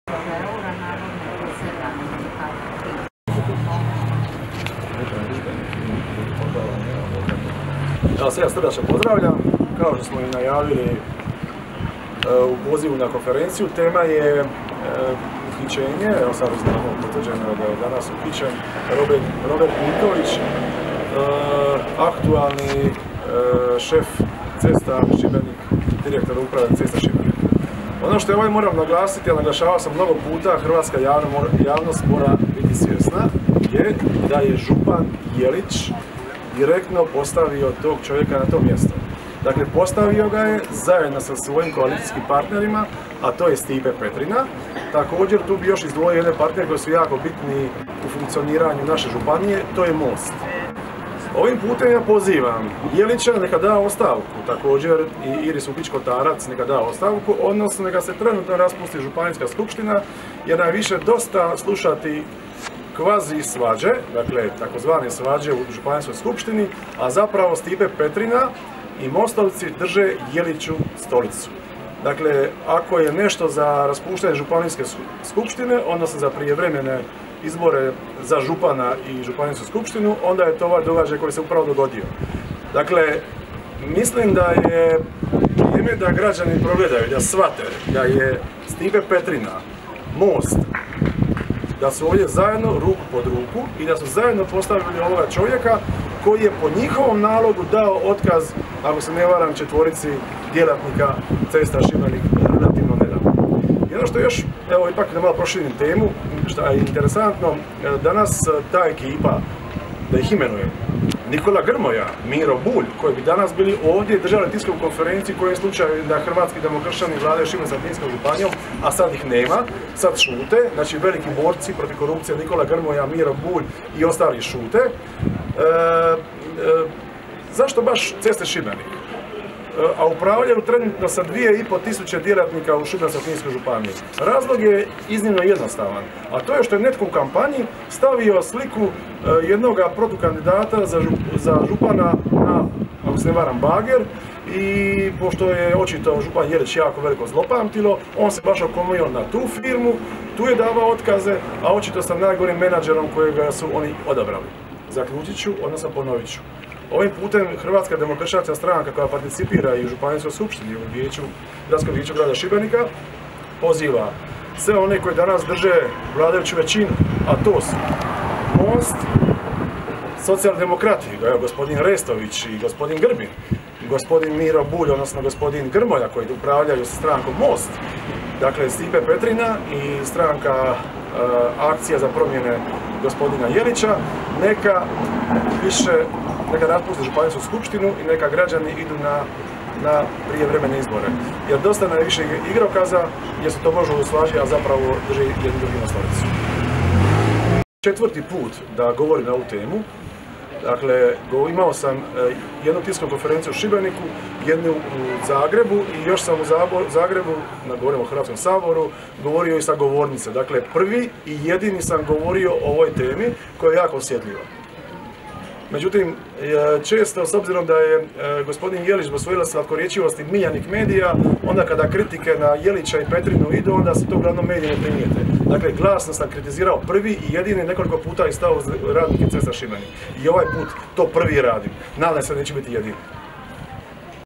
Zekanović: "Pozivam župana Jelića na podnese ostavku i raspusti županijsku skupštinu"
Saborski zastupnjik Hrvoje Zekanović održao je konferenciju za medije ispred zgrade županije. Tema konferencije bila je Je li Miljković,direktor Ceste Šibenik, samo "Pedro" ili postoji politička odgovornost onih koji su ga postavili.